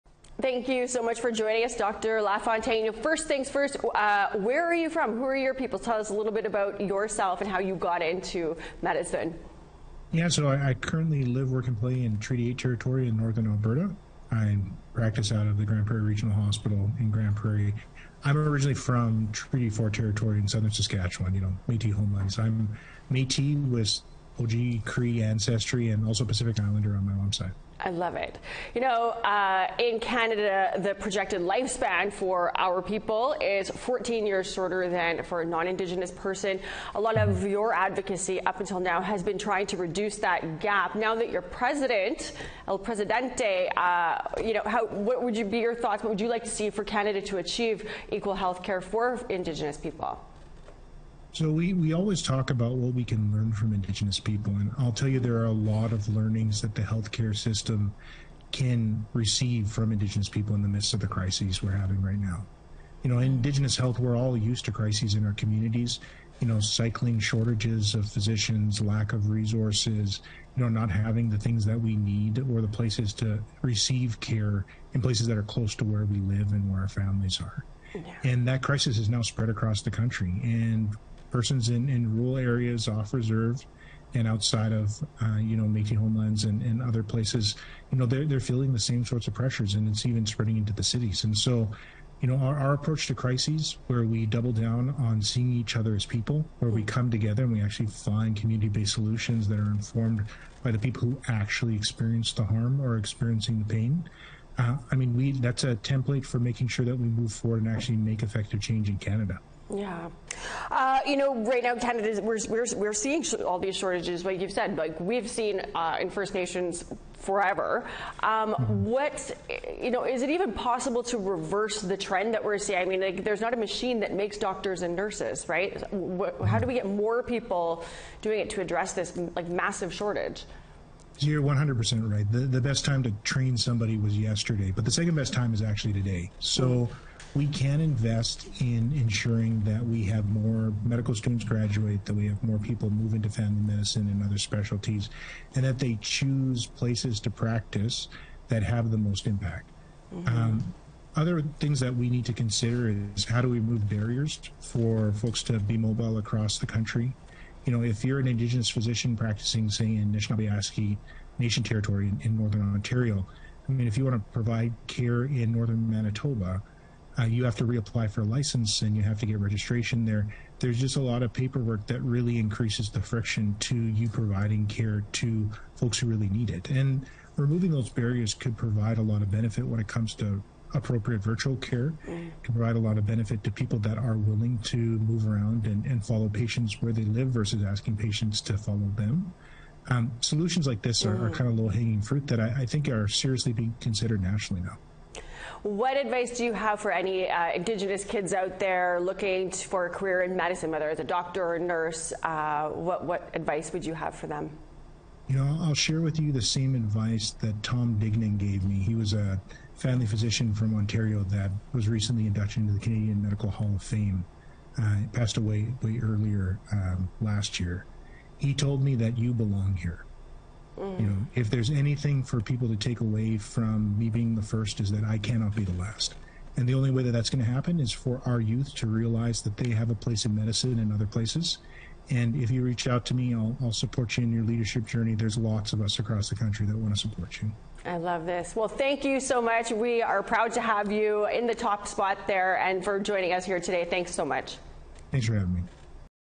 First Indigenous president of the Canadian Medical Association starts term | APTN News
The new head of the Canadian Medical Association starts his term today as the organization's first Indigenous president.

At 40 years old, Dr. Alika Lafontaine is also the youngest to lead the CMA.